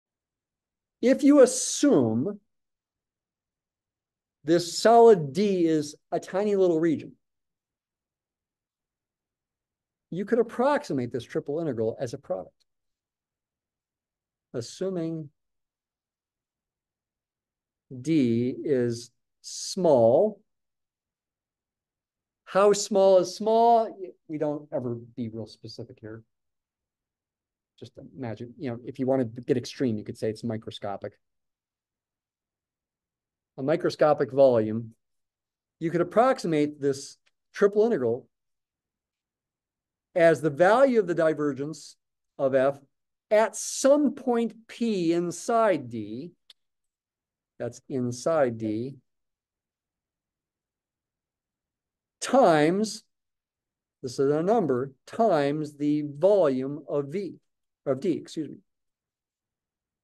TRIPLE INTEGRAL of DIVERGENCE Over a Microscopic Volume? Here's the Trick...
In vector calculus, evaluating a triple integral of divergence over a very small (even microscopic) solid region lets you approximate the result in a surprisingly simple way: just compute a product! This Short breaks down how the divergence of a vector field over a tiny domain D can be approximated as div(F)(P)*vol(D), where P is a point inside D.

🧠 Great for students in multivariable calculus, physics, and engineering!

📖 Visual Complex Analysis, by Tristan Needham